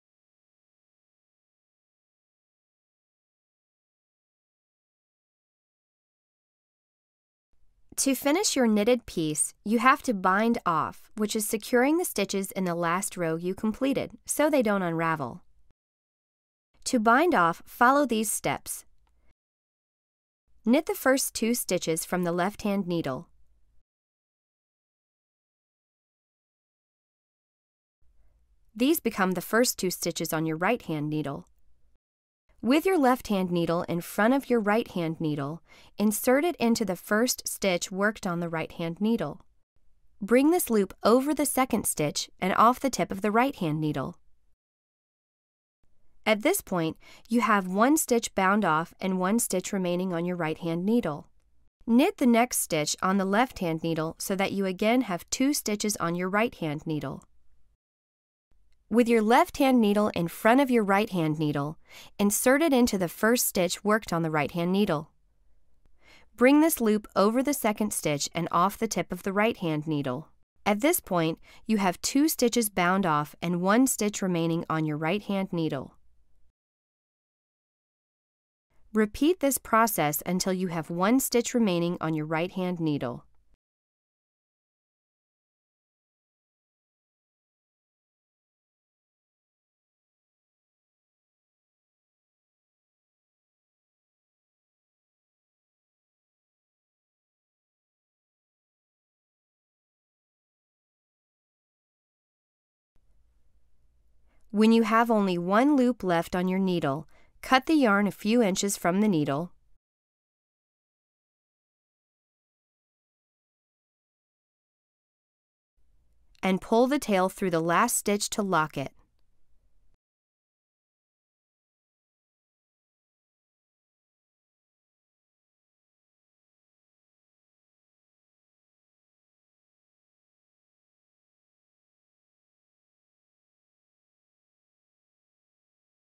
How to Bind Off Your Knitting For Dummies
Finish your knitted piece by binding off (or casting off) to secure the stitches in the last row you've completed. This video shows you how to bind off to ensure that your knitting doesn't unravel.